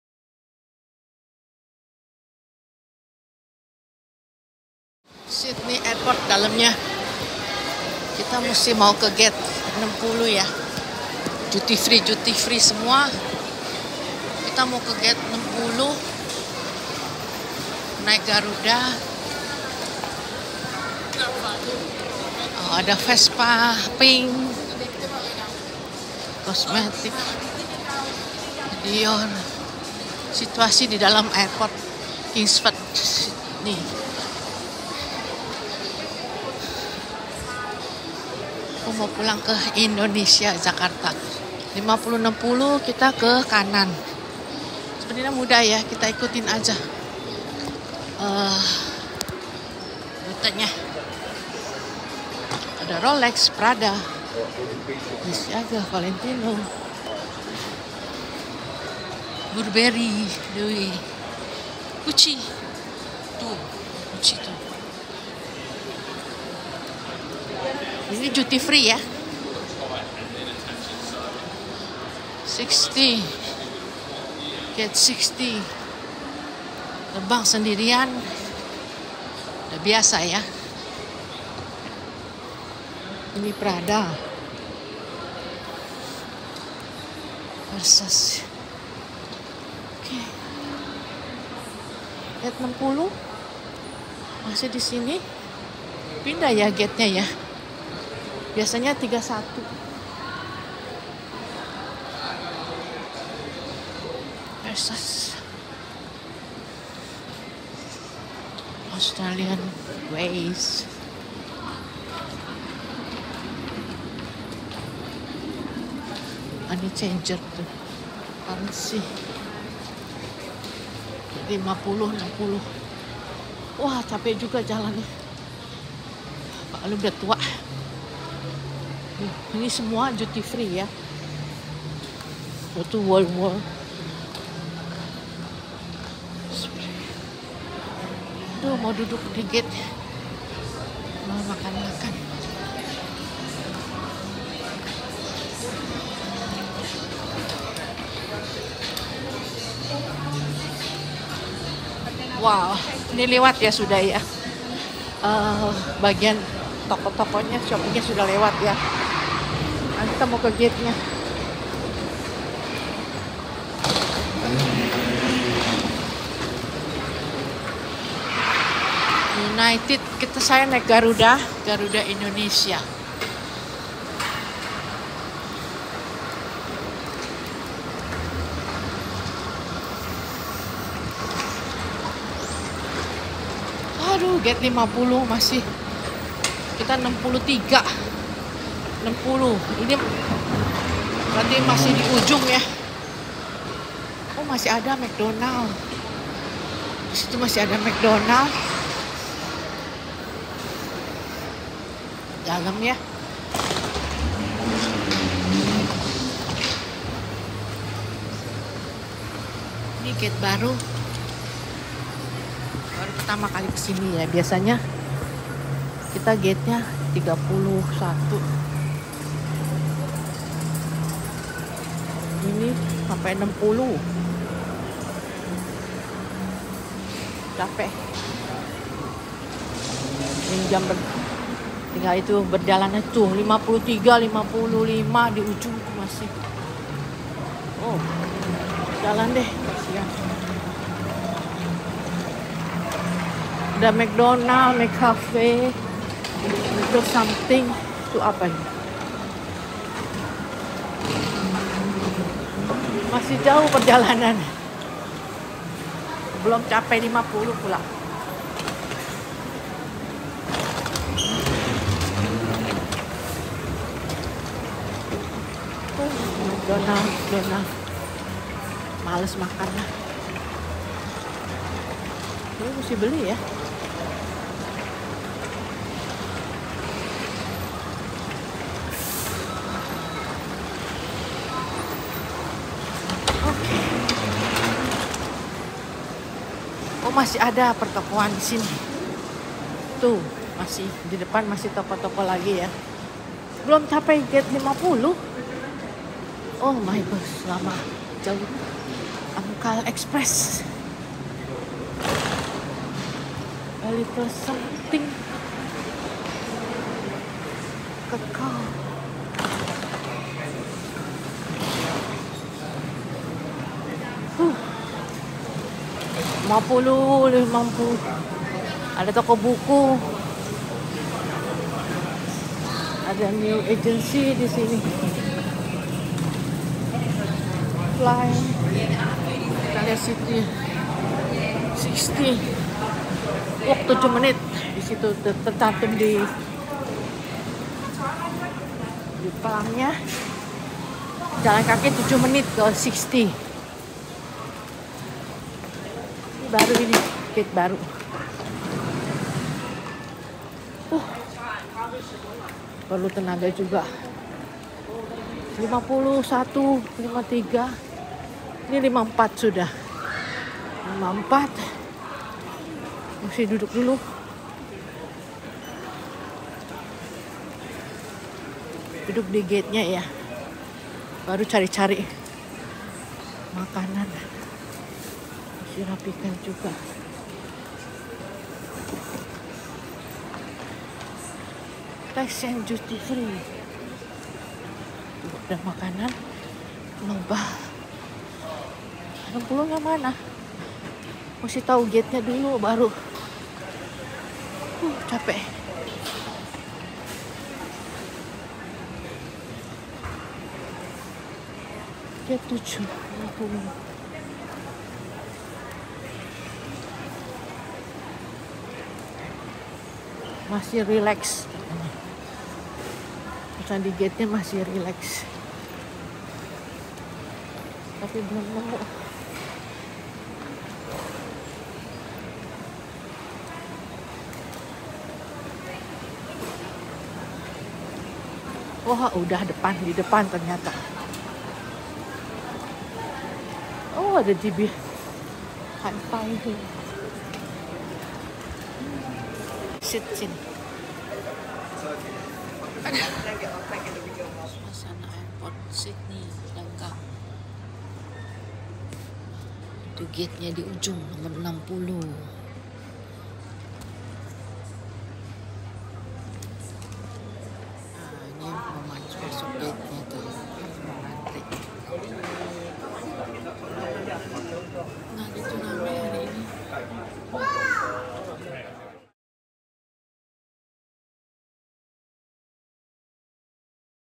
My Journey to Sydney International Kingsford Smith Airport 25 October 2023
Garuda 0713 flight Sydney to Jakarta Indonesia

Walk to gate 60 , I think this is the furthest gate ( new gate ? )
So tired
Not so crowded

I have to wait several hours  because too early come to the airport